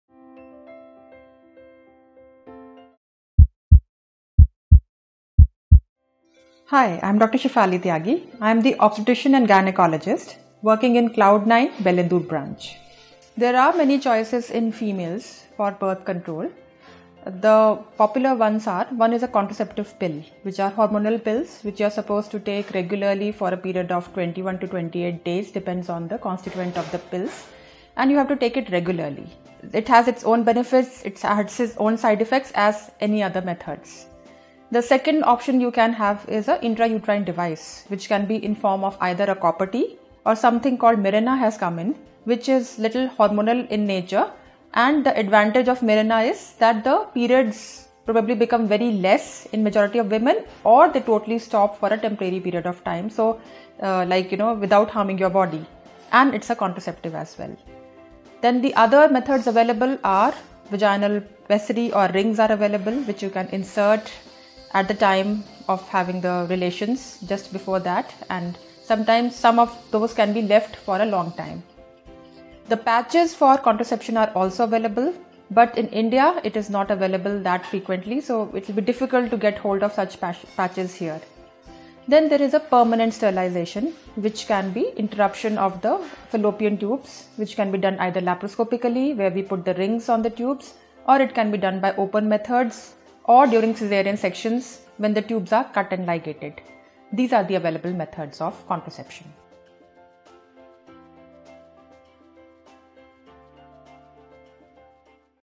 Different Birth Control methods in females - Dr. Shefali Tyagi
There are many choices in females for birth control. The  popular ones  the  contraceptive pills  which are hormonal pills that is taken  regularly  for  a period  of  21  to 28  days  depending on the  constituent  of the pills  and you have  to take it  regularly. It has its own side effects and benefits as in any other methods. The  second option you can have is an intrauterine device,   either in the  form of  a  Copper T  or  something called   Mirena  has come in which is little hormonal in nature and  the advantage of MIRENA is  the cycles  become  very less or in majority of  women  it  initially  stops for  a  temporary period of time without harming your body. So it is like a contraceptive. Then the other methods available are pessary or rings are available which you can insert at the time of having relations and some of those  can be left for  long time.  The patches are available but in India it is not available that frequently . So it will be difficult to get hold of that. Then there will be permanent sterilisation that causes disruption of fallopian tubes which can be done enter laparoscopically where we can put rinsing on the tubes, it can be  done by  open methods or by caesarean section here the tubes are  cut and ligated. These are the available methods of contraception.